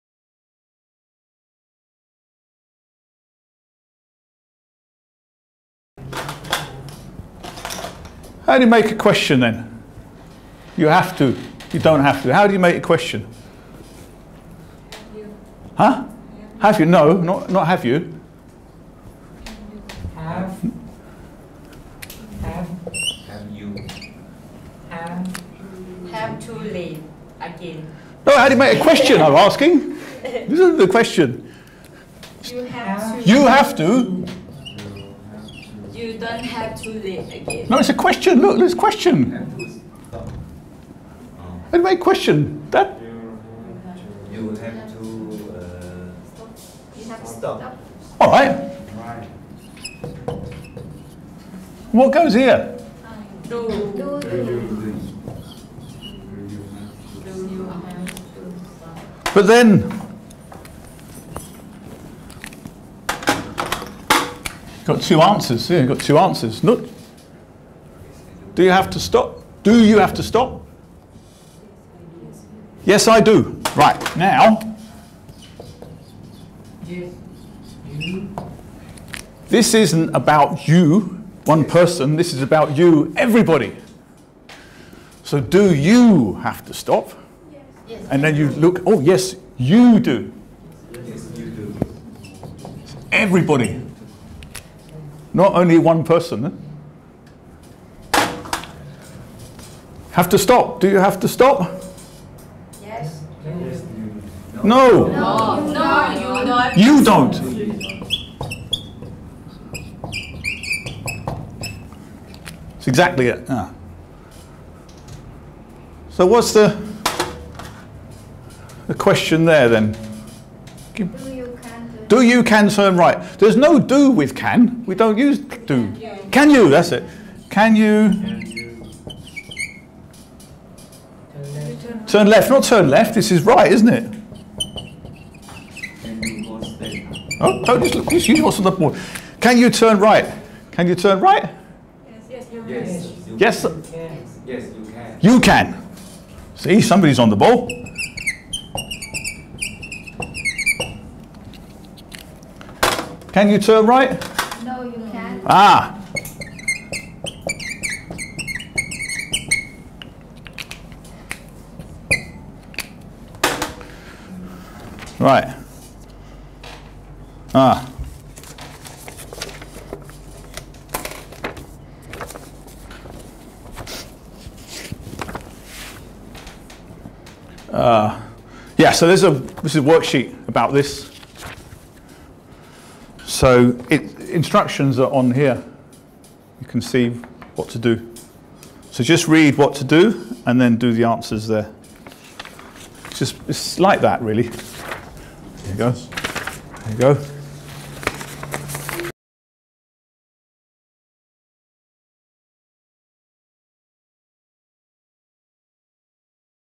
EFL Sample Lesson – Modal Verb Questions
This video is a sample EFL lesson in a real-life classroom setting abroad. The teacher elicits vocabulary from the students and begins with the study phase of the lesson. The focus of this lesson is modal verbs and how to form questions using the modal verbs ‘have to’ and ‘can’. It is important to write this information on the board in clear writing for the students to be able to understand and copy it down.